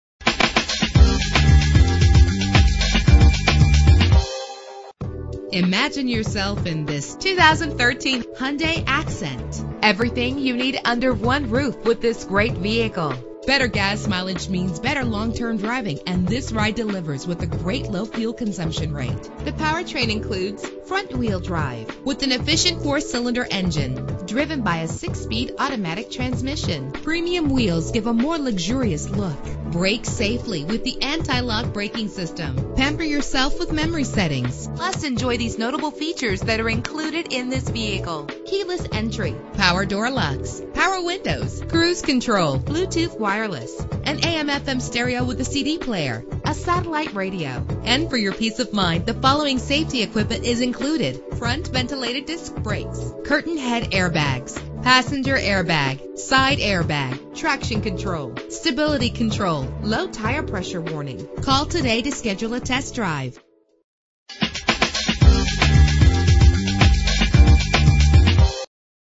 2013 Hyundai Accent - Daphne AL
Year: 2013
Make: Hyundai
Model: Accent
Trim: SE
Engine: 1.6 liter inline 4 cylinder 16 valve
Transmission: 6-Speed Automatic
Color: Marathon Blue
Mileage: 5
Address:
29736 Frederick Blvd.